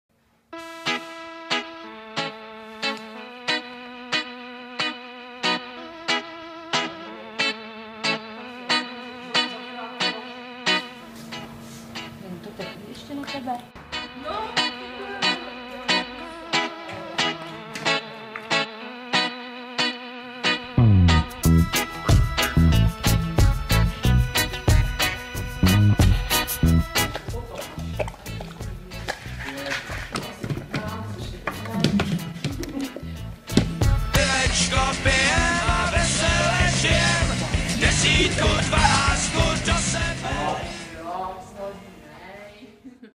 Pozvěte svého psa do restaurace
Liberec má další české prvenství, restauraci zaměřenou výhradně na čtyřnohé zákazníky... více zde